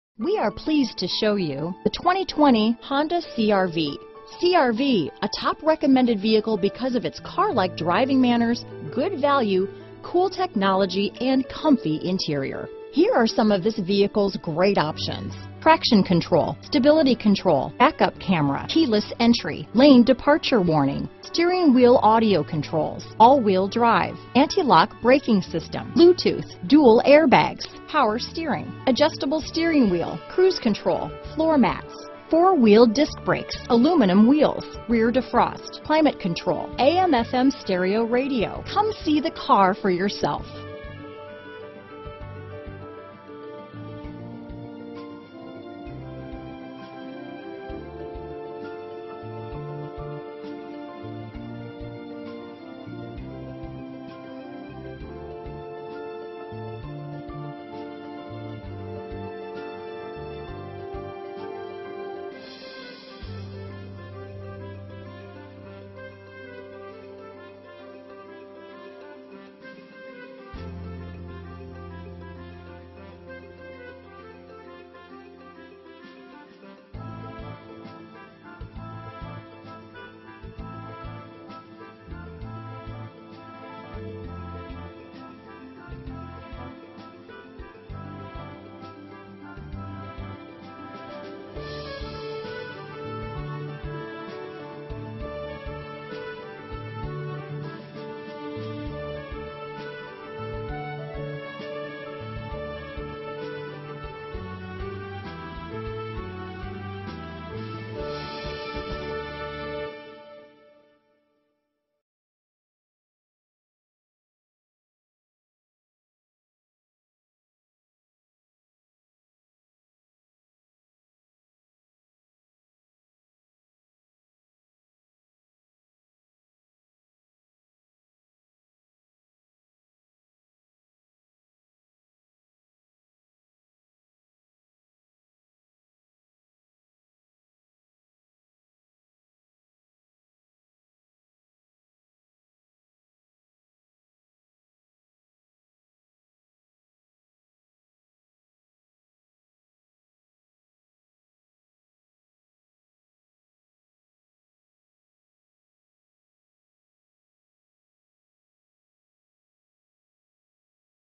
2020 Honda CR-V Aurora, Denver, Highland Ranch, Parker, Centennial, CO 43449
Radiant Red New 2020 Honda CR-V available in Aurora, Colorado at

Boasts 32 Highway MPG and 27 City MPG! This Honda CR-V delivers a Intercooled Turbo Regular Unleaded I-4 1.5 L/91 engine powering this Variable transmission. Wheels: 17 Alloy, Valet Function, Urethane Gear Shift Knob.*This Honda CR-V Comes Equipped with These Options *Trip Computer, Transmission: Continuously Variable w/Sport Mode, Tires: 235/65R17 104H All-Season, Tailgate/Rear Door Lock Included w/Power Door Locks, Systems Monitor, Strut Front Suspension w/Coil Springs, Steel Spare Wheel, Sliding Front Center Armrest and Rear Center Armrest, Single Stainless Steel Exhaust, Side Impact Beams.* Stop By Today *Stop by Mile High Honda located at 2777 S Havana St, Aurora, CO 80014 for a quick visit and a great vehicle!